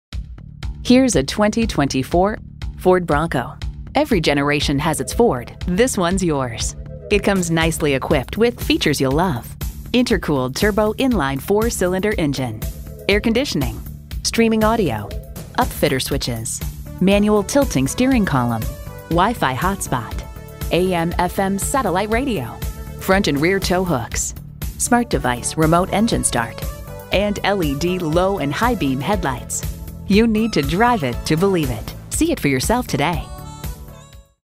Used 2024 Ford Bronco Elizabeth City, NC 8249188B - SOLD
Year: 2024
Make: Ford
Model: Bronco
Trim: Badlands
Engine: 2.7L EcoBoost V6
Transmission: 10-Speed Automatic
Color: Red
Mileage: 3349
Stock #: 8249188B
VIN: 1FMEE9BP5RLA32293

Address:
1310 N Road Street (Business Route 17)